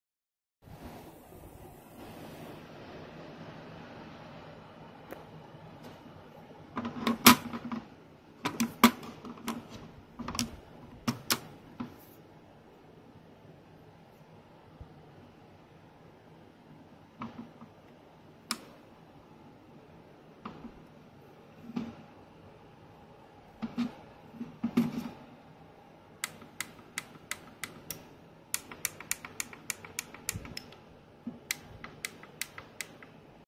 Led Travor 120 TL-120S not working
The light keeps blinking and dodsnt let to change the power intensity of the light